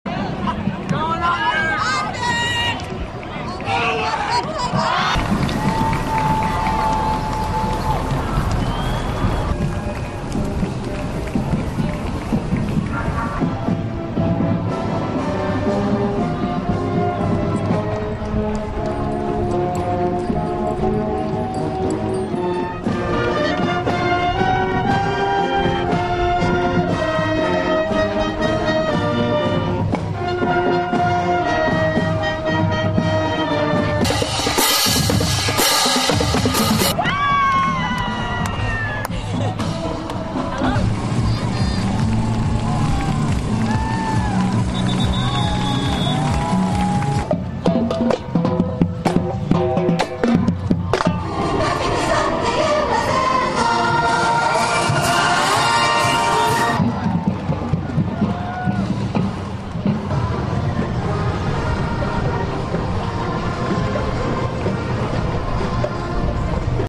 All-American Parade in Nation's Capital
The Fourth of July in Washington, D.C. is marked by a parade down the capital's promenades. (July 4)